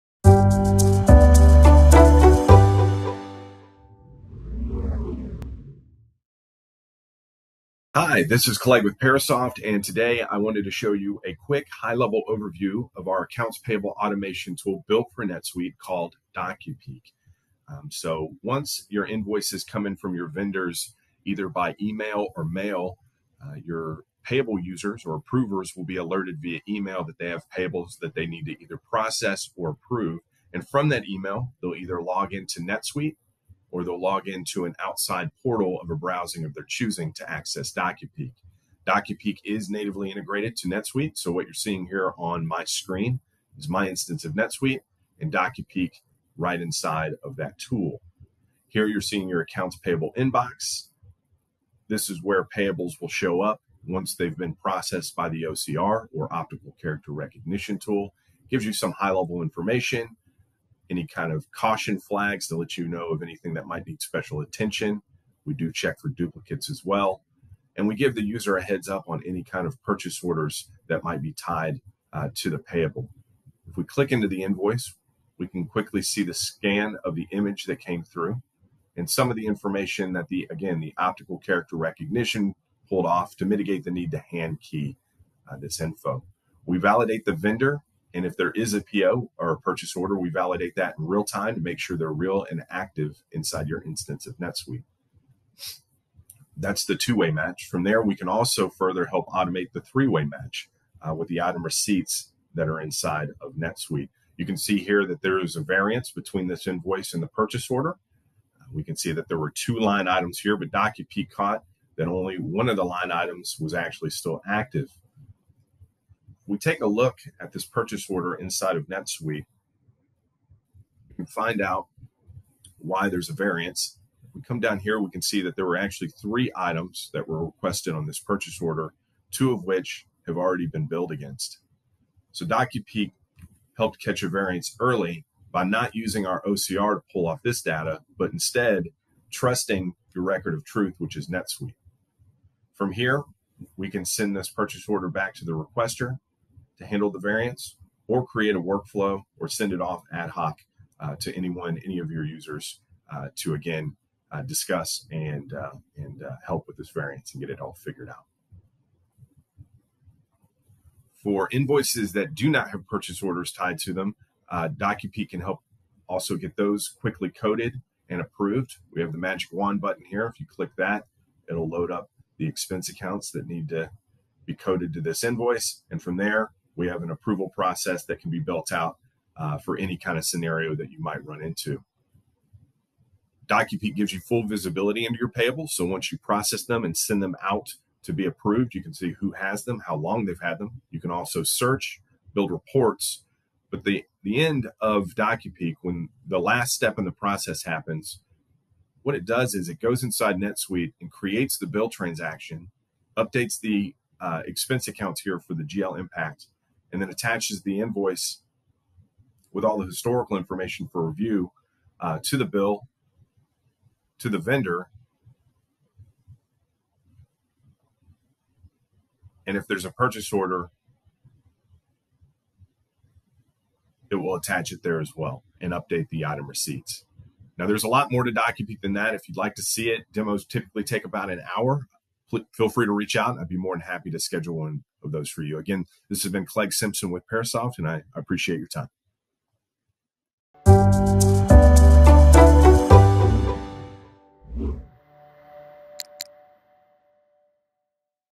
Demo: AP Automation for NetSuite
Process over 4x the number of paper invoices per day, per employee. PairSoft seamlessly blends into NetSuite's interface to work in concert with your current database. Our software eliminates manual data entry associated with invoicing, audit trails, purchase orders, and approval workflows